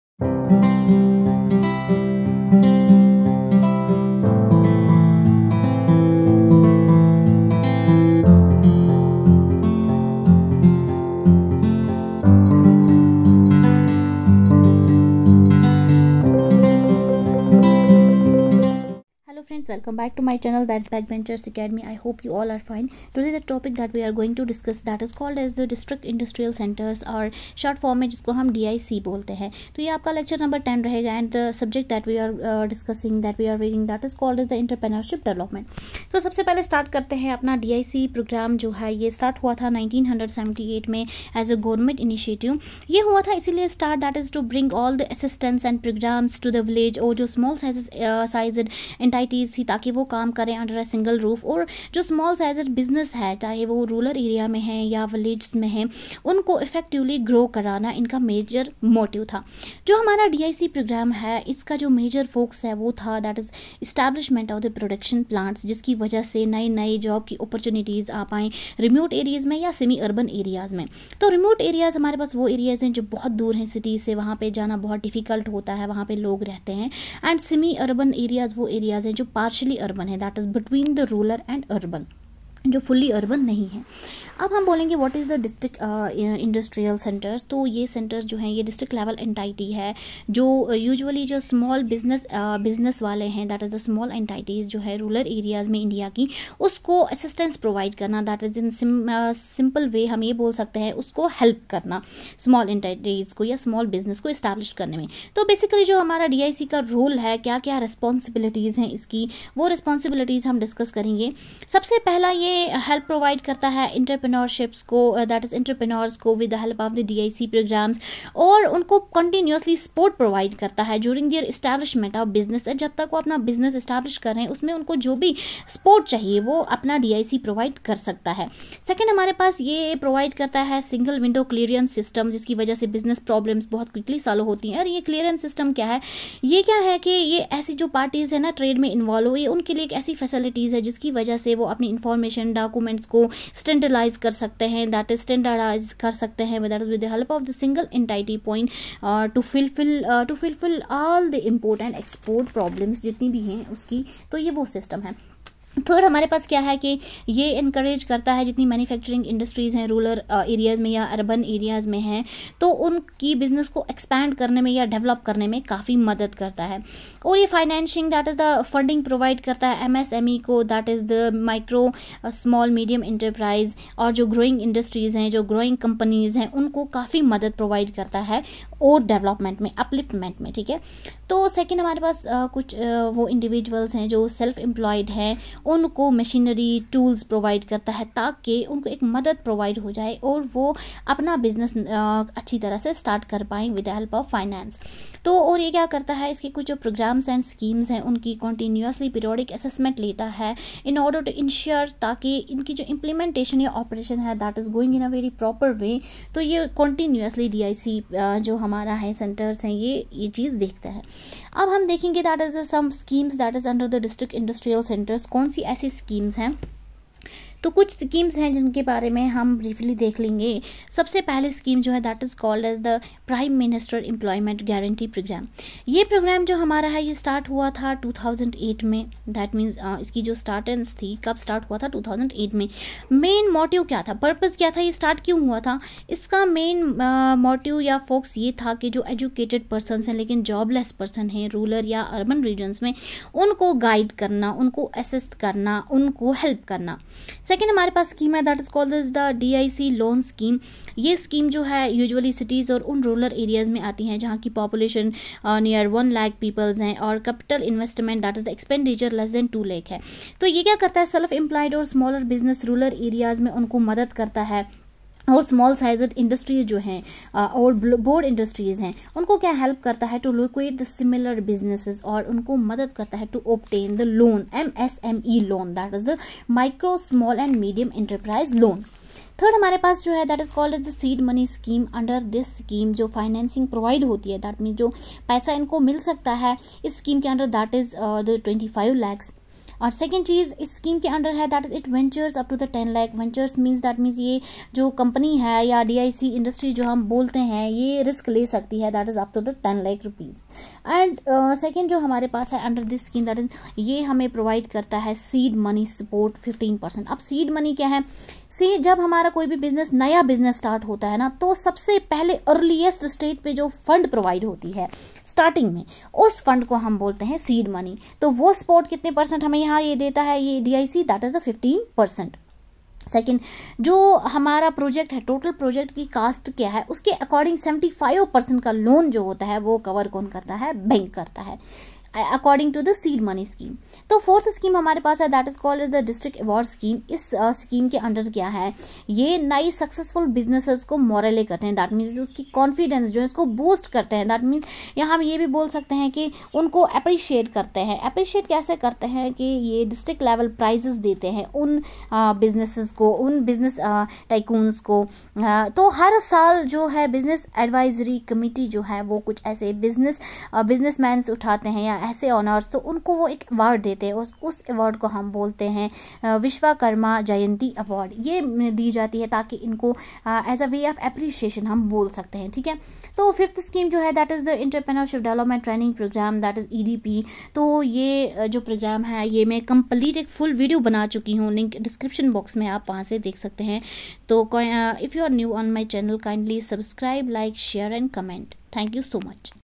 District Industrial Centers (DIC) (Entrepreneurship Lecture -10)
In this video You learn all about District Industrial Centers like what are DIC / What are their functions .
I discuss the following topics :
- Introduction To DIC
- Why DIC
- what is  DIC
- Role of DIC
- schemes under DIC
* Prime minister employment guarantee program
* DIC Loan scheme
* Seed money scheme
* District Awards scheme